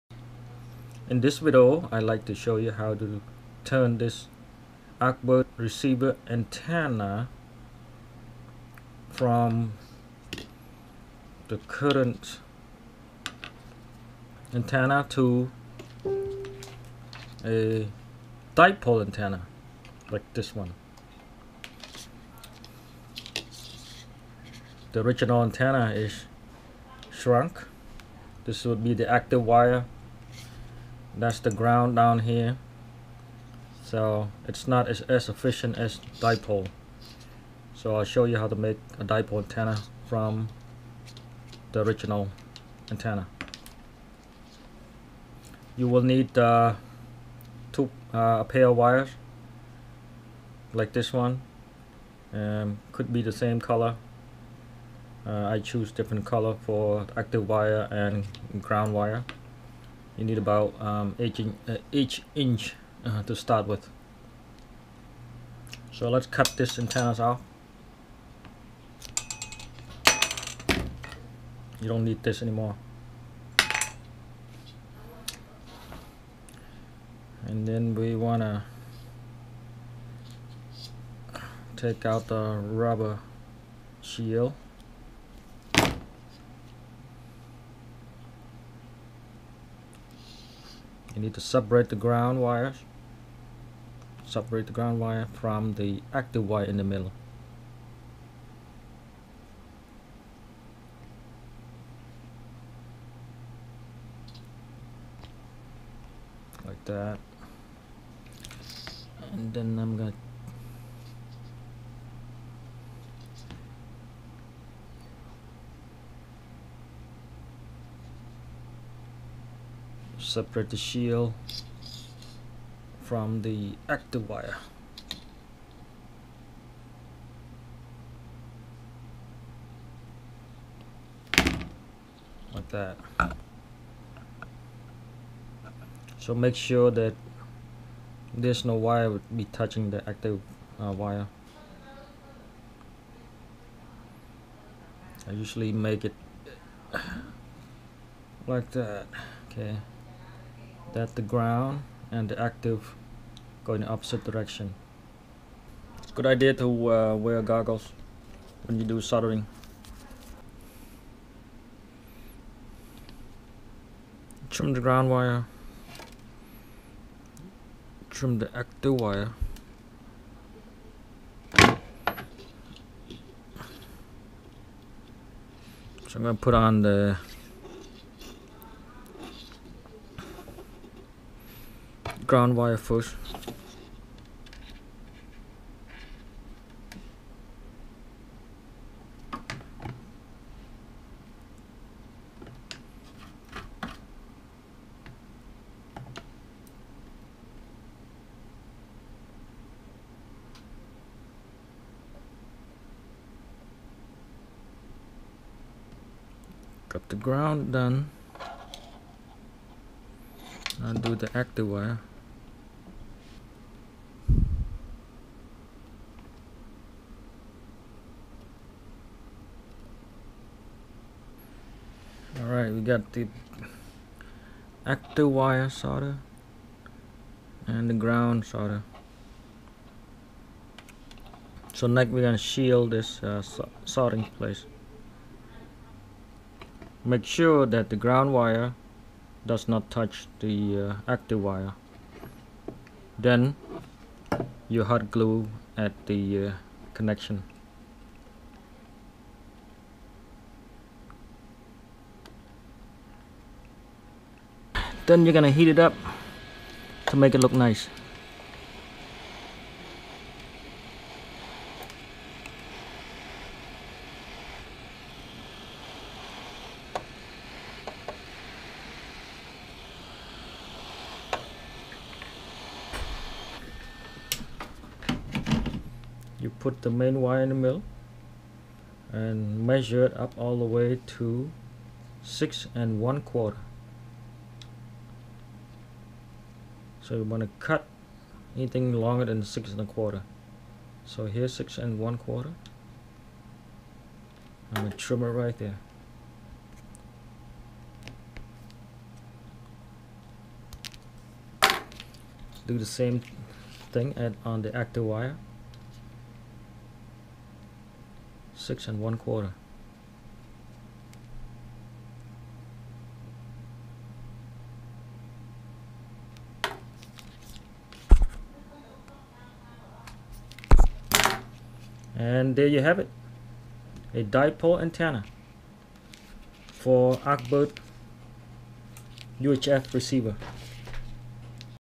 How To make Dipole Antenna for Arkbird UHF 433Mhz Receiver
Use a solid copper coded 18 gauge wire, not strings of wire.
A dipole antenna is more efficient than the original Arkbird antenna.  It will give you more range.